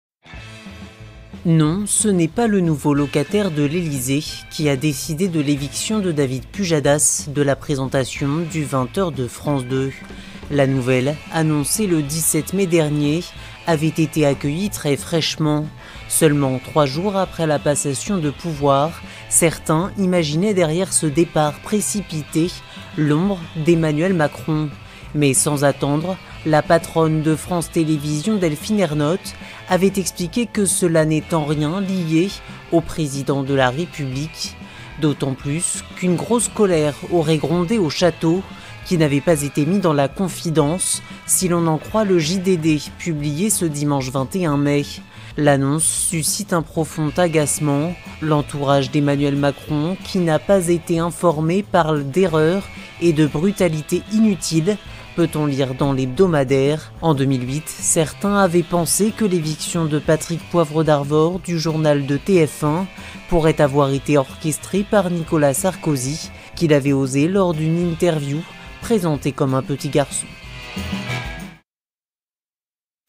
L'Elysée très agacé par l'éviction de David Pujadas
Non ce n'est pas le nouveau locataire de l'Elysée qui a décidé de l'éviction de David Pujadas de la présentation du 20 Heures de France 2. La nouvelle annoncée le 17 mai dernier avait été accueillie très fraîchement. Seulement trois jours après la passation de pouvoir, certains imaginaient derrière ce départ précipité l'ombre d'Emmanuel Macron. Mais sans attendre, la patronne de France Télévisions Delphine Ernotte avait expliqué que cela n'est en rien "lié au nouveau président de la République". D'autant plus qu'une grosse colère aurait grondé au château qui n'avait été mis dans la confidence si l'on en croit le JDD publié ce dimanche 21 mai. "L'annonce suscite un profond agacement. L'entourage d'Emmanuel Macron, qui n'a pas été informé, parle d'"erreur" et de "brutalité inutile"", peut-on lire dans l'hebdomadaire. En 2008, certains avaient pensé que l'éviction de Patrick Poivre d'Arvor du journal de TF1 pourrait avoir été orchestrée par Nicolas Sarkozy qu'il avait osé lors d'une interview présenter comme un "petit garçon".